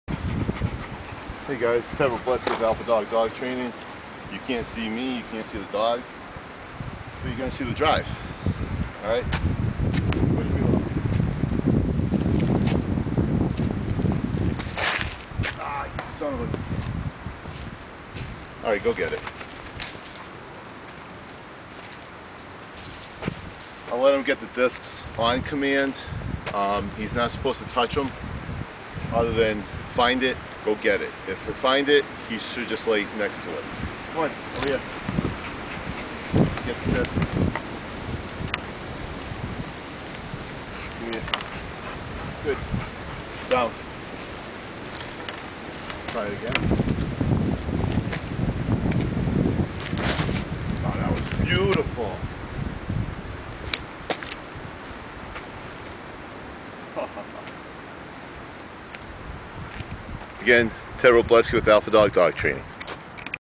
Kona is understanding the rules
Bad drive from the Tee and Kona staying in the down command until I have him "go get it".  He retrieves the disc for an awesome drive from the Tee, thank you e-collar from e-collar technologies